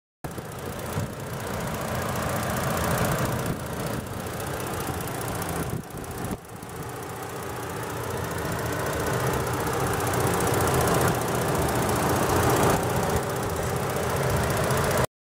GovDeals: 2014 Ford Explorer Police SUV AWD (ID #7485) (E0)
2014 Ford Explorer Police SUV AWD  (ID #7485) (E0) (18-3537 WSP) sold by State of Washington Surplus Operations, WA on GovDeals.com